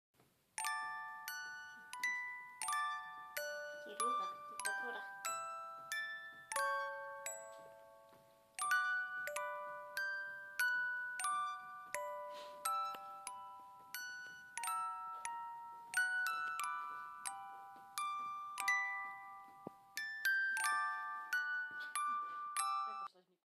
#9 - Bábätko!
Išli sme navštíviť rodinu a hľa, kto si ľahol ku Kire na postel... (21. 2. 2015).